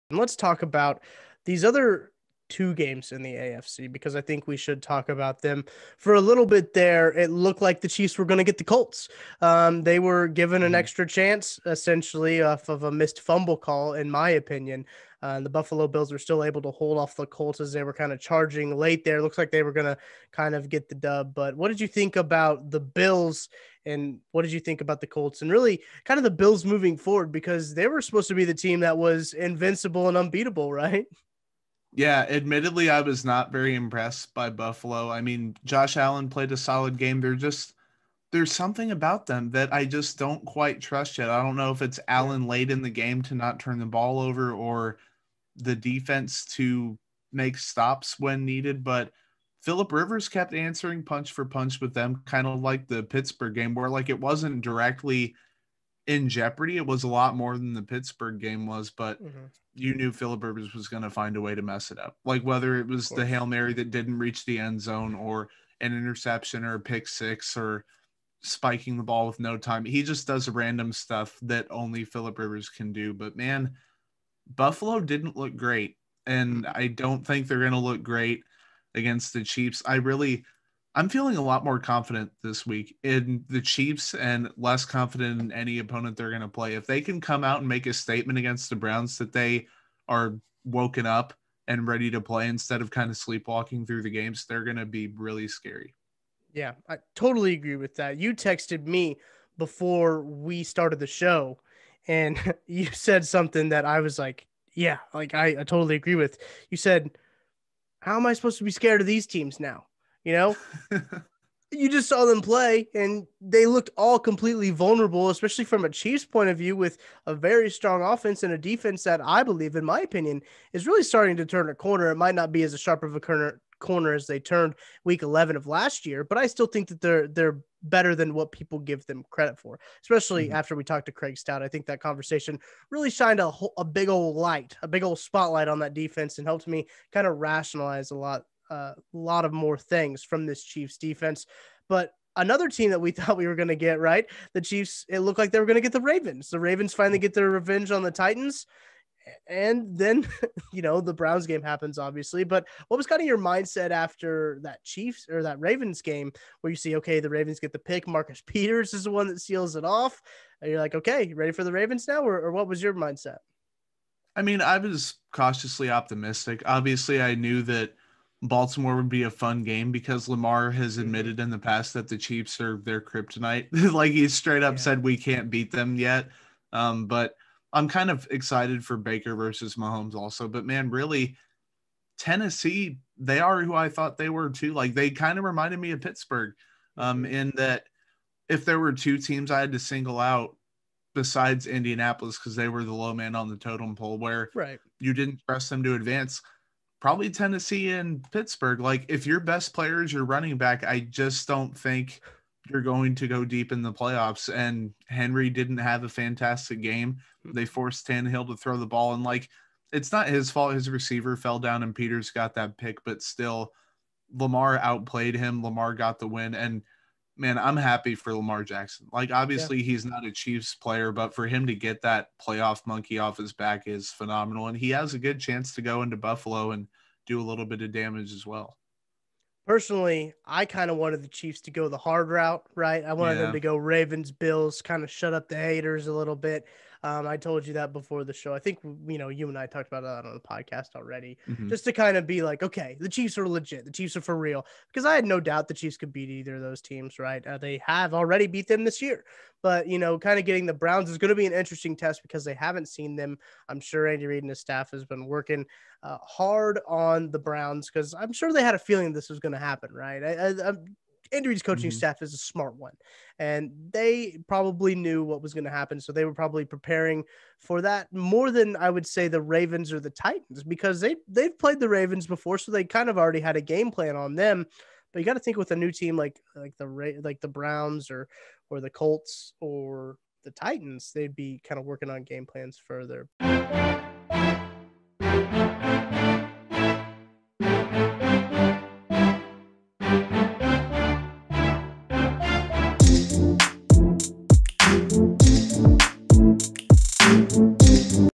Assessing the AFC After Super Wild Card Weekend
Arrowhead Report's Tucker Franklin and Jordan Foote give their first reactions about the Kansas City Chiefs' first-round matchup with the Cleveland Browns and assess what happened in the AFC following NFL's Super Wild Card Weekend.